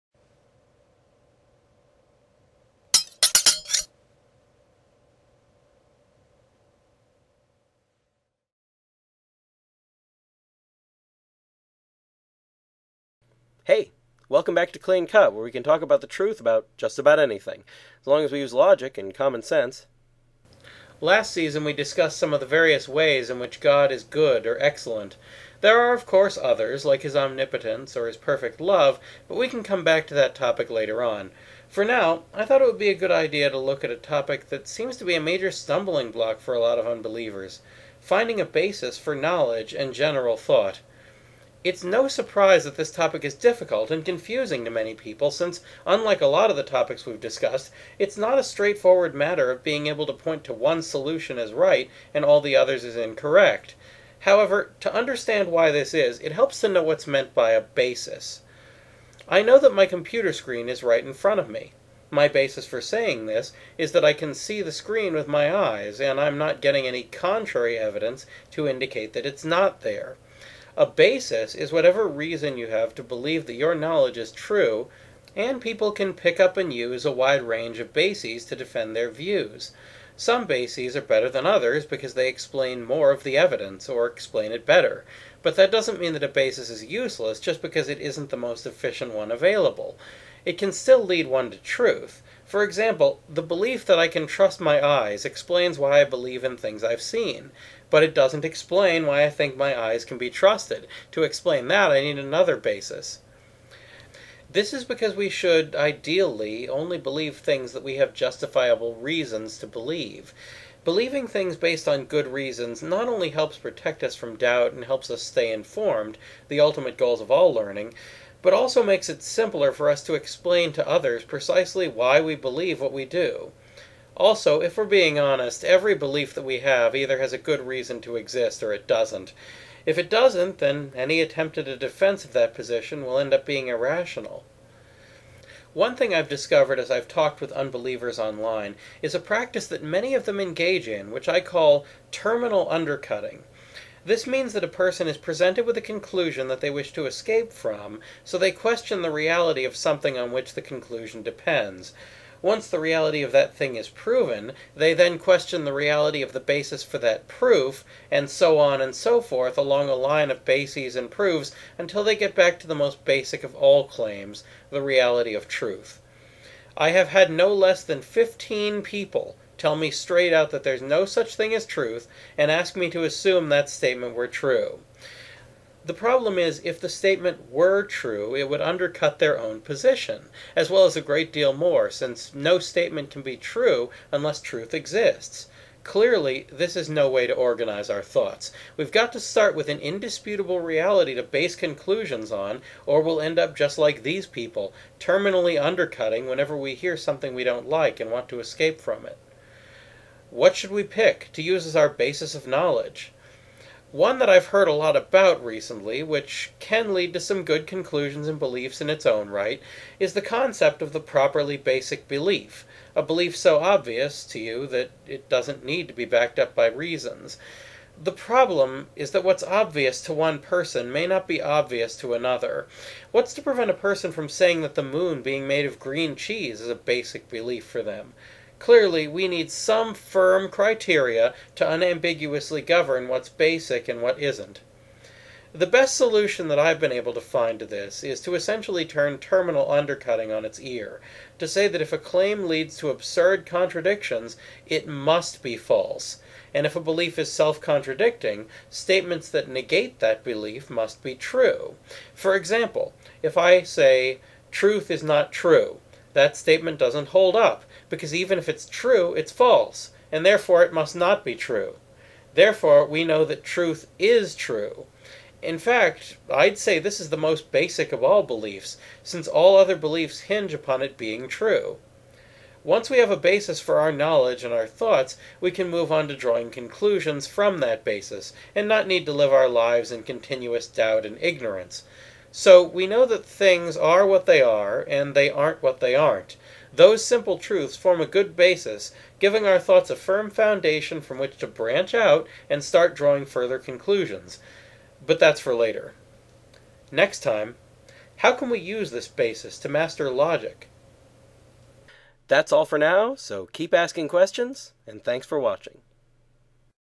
Can Our Thoughts Have a Firm Basis?
Why should we have a basis for our thoughts?  What is terminal undercutting?  How can we use simple truths to escape from terminal undercutting?